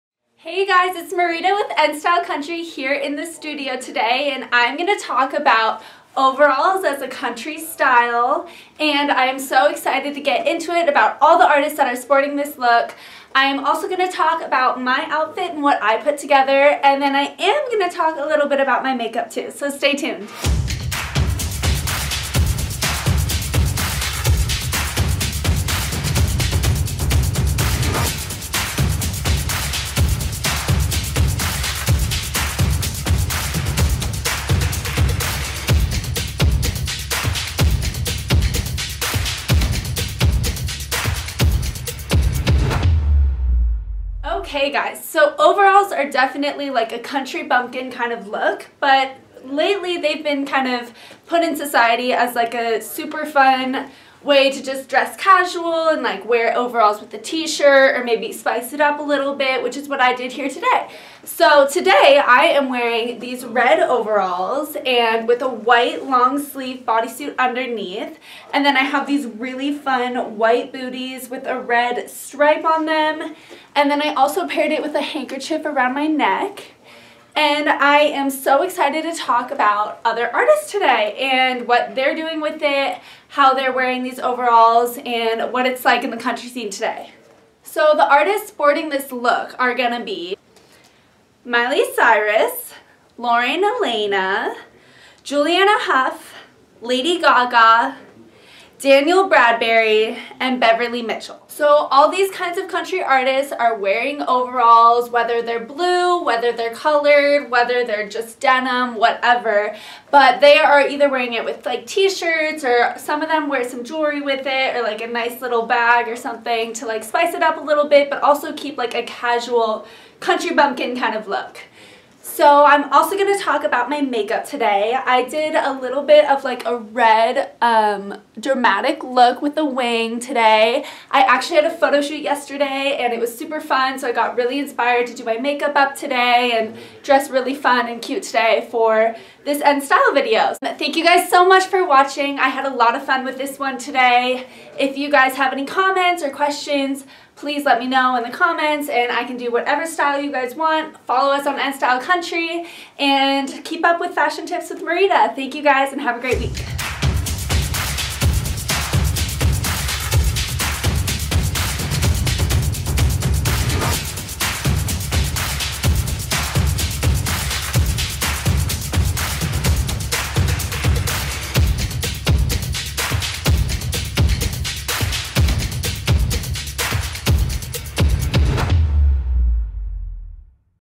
Nstyle OOTD Overalls!!
NStyle Country fashion tips of the day with Marita brings you style pointers on country bumpkin overalls!